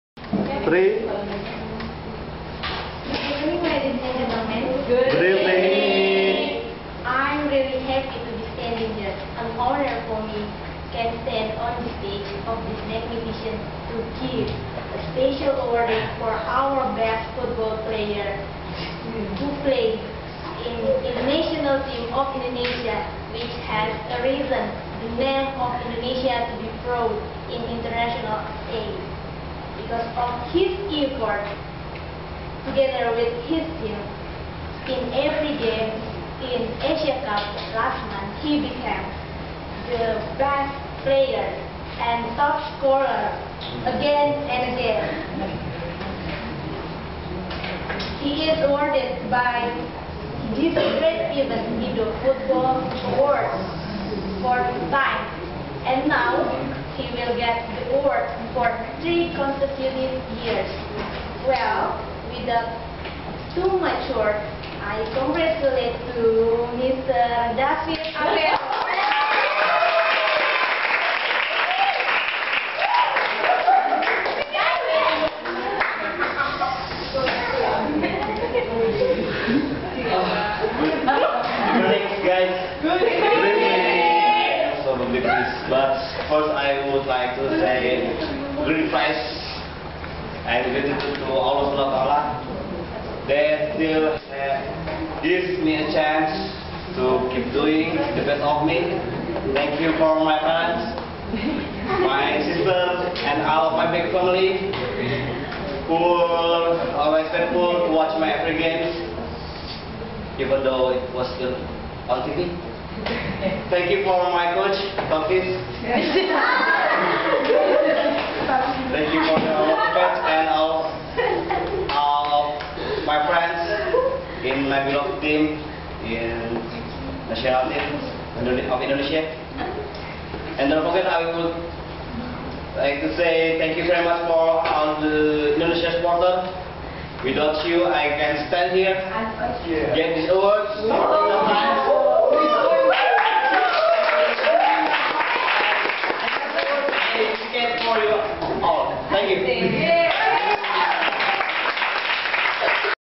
Speech of Presenting and Acceptance-David A. & Lydia R.
Lydia: Your speech is okay yet the delivery needs some improvements. First, you often look at the left side of your audience. It is clear from they position of your head. When you address your audience, you have to give equal attention to all sides of your audience. You can spend 1 minute looking at the left, 1 minute for the center part, and another minute to the right. David: Your acceptance speech is not convincing enough. You should show excitement because it shows that you appreciate the award. So, do not show flat expression. Try to be more relax and just be loose for speech like this.